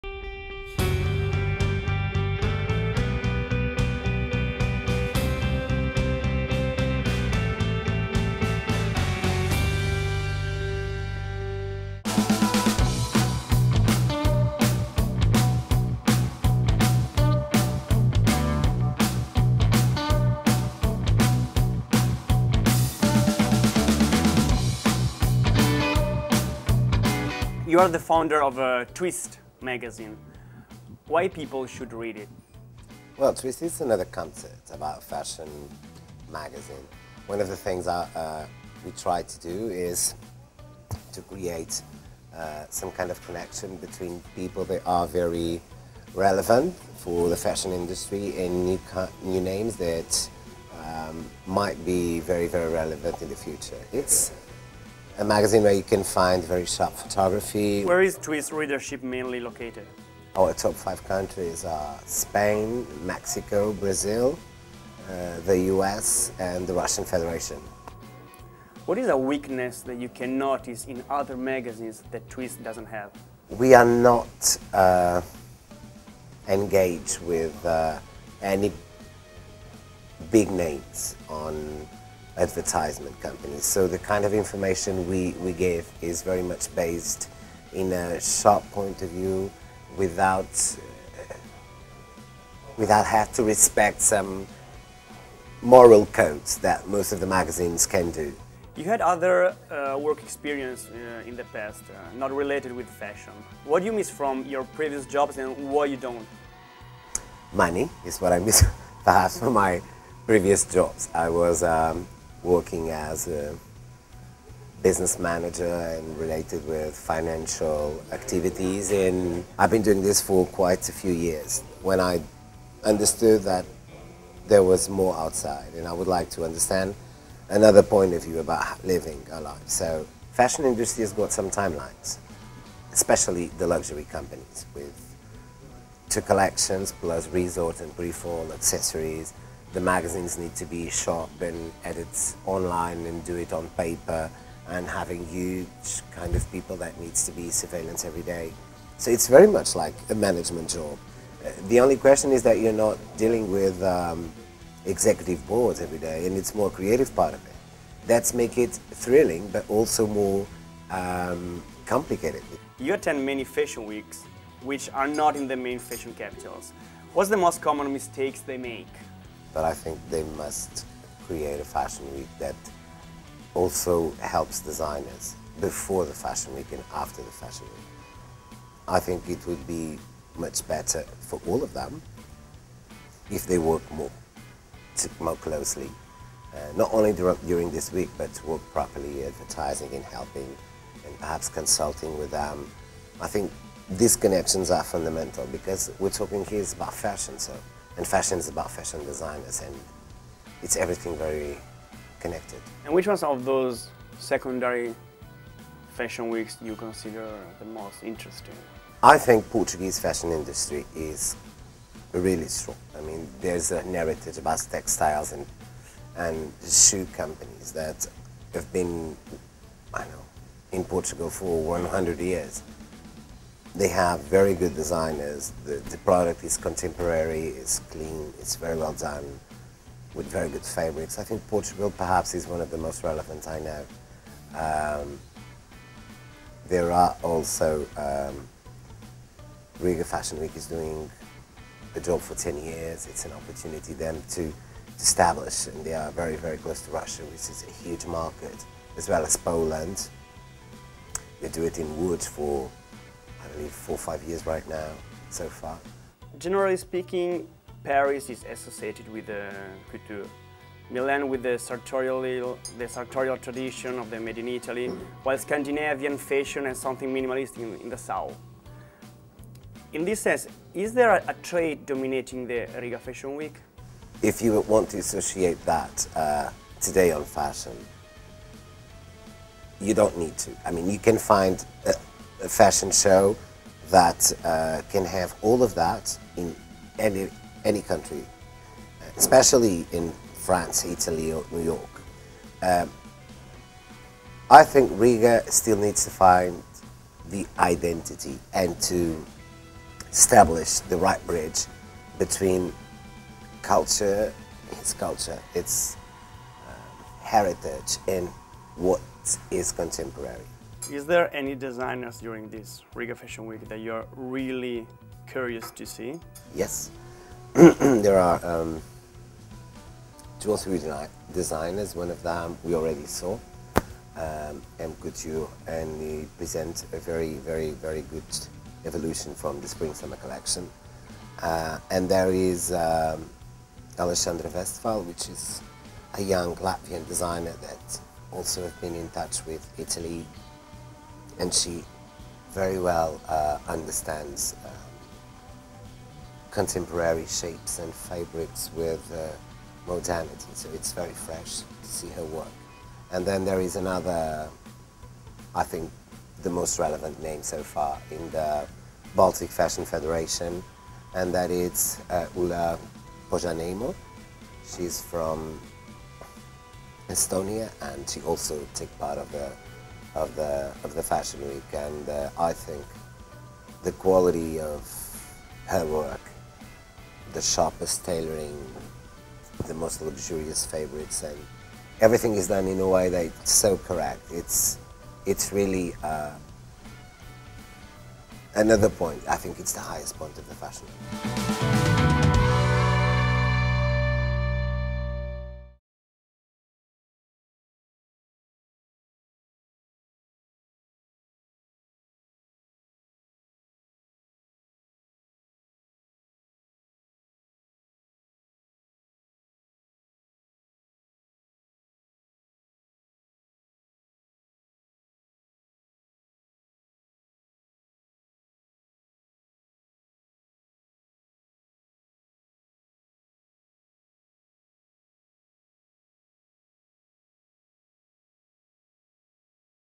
Design42Day - Rumors with Mr. Norbert 1/2
Take a look at the interview with the founder of Twissst Magazine Norberto Lopes Cabaço recorded during the Riga Fashion Week AW 2014/15 at the Darzs Restaurant.